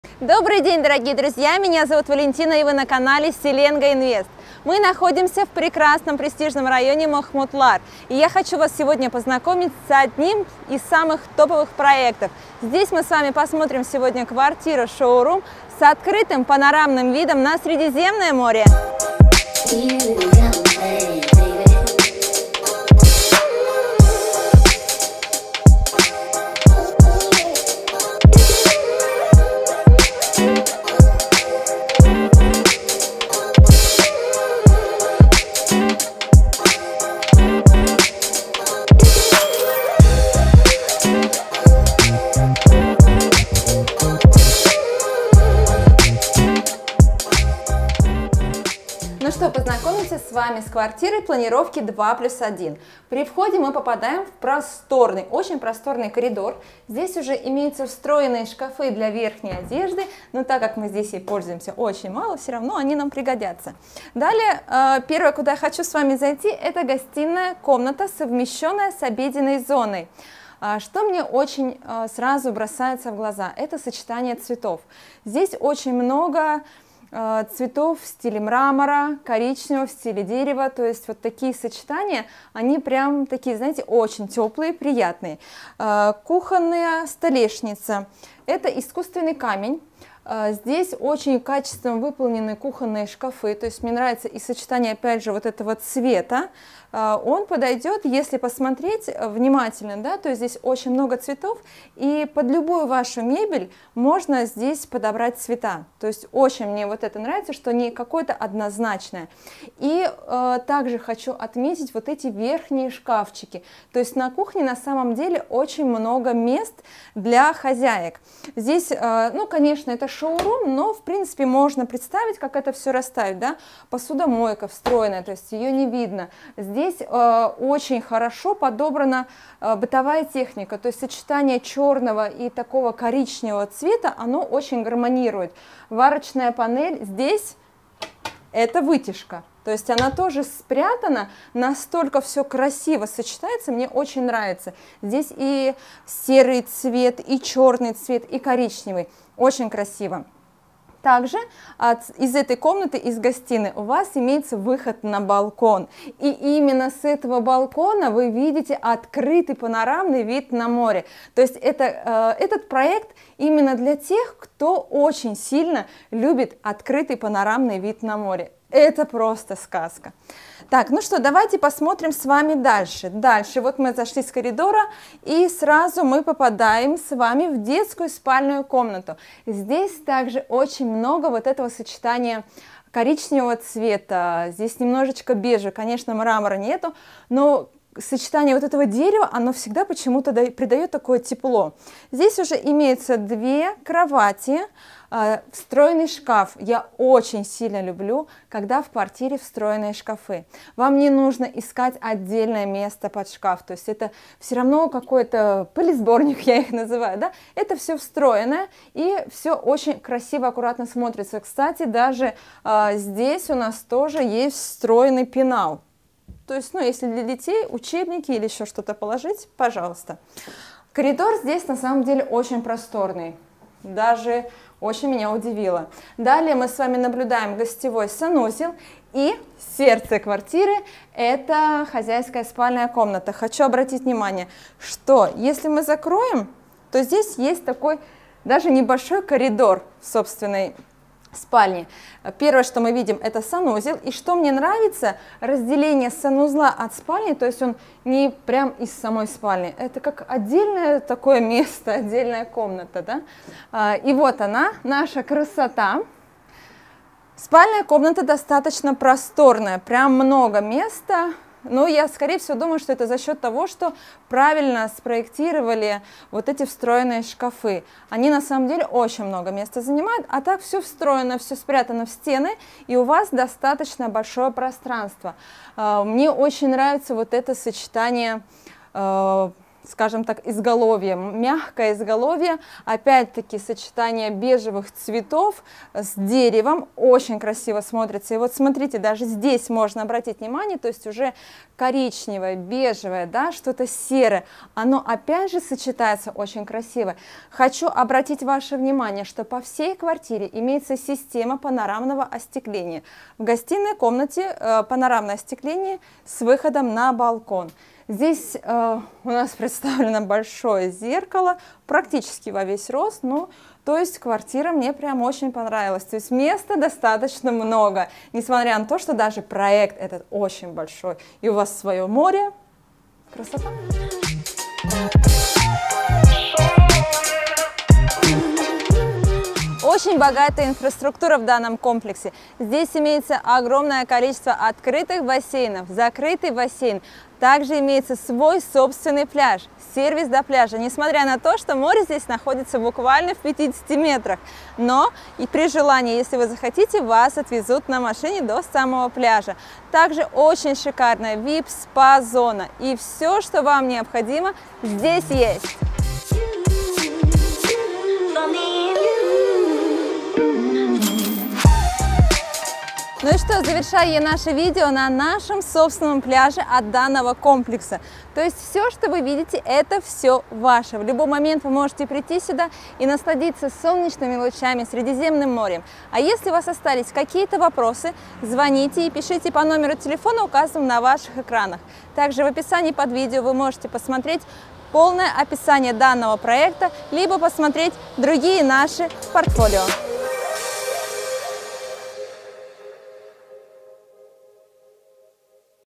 Новый проект на центральном побережье Махмутлара | Недвижимость в Алании
Сегодня мы хотим рассказать вам о потрясающем новом проекте недвижимости в Алании 🌴

Что делает этот проект настолько особенным❓ Прежде всего, его идеальное месторасположение - он находится в самом центре города и предоставляет уникальные преимущества. Подземный переход прямо к пляжу обеспечивает комфорт и безопасность для всех жильцов.

Помимо этого, проект имеет собственный частный пляж и находится в непосредственной близости от популярной береговой линии, усыпанной разнообразными туристическими заведениями и развлечениями. Это идеальное место для тех, кто желает наслаждаться морем, одновременно находясь в центре городской активности.

С этим проектом вы сможете открыть для себя совершенно новый мир, который сочетает в себе морское удовольствие и урбанистическую жизнь!